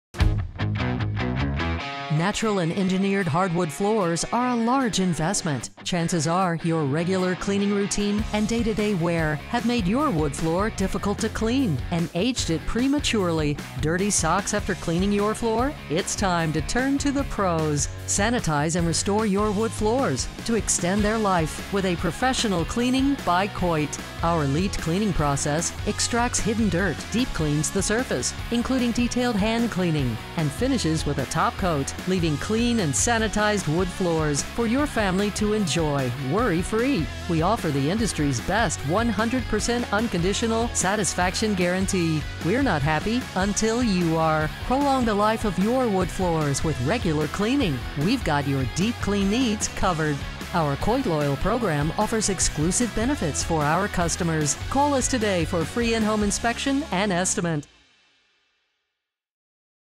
COIT Cleans Wood Floors
At COIT, have specialized expertise to keep your hardwood floors looking shiny, clean, and like new. Extend the life of your hardwood floors with professional cleaning. Our advanced cleaning methods remove deeply embedded dirt and debris that erode hardwood floors. Our protective finish will give your floors a streak-free shine! Our elite cleaning process features specially formulated products and state-of-the-art cleaning methods for your hardwood floors. Our services are protected and backed by our 100% unconditional satisfaction guarantee.

Learn more and book a free in-home inspection and estimate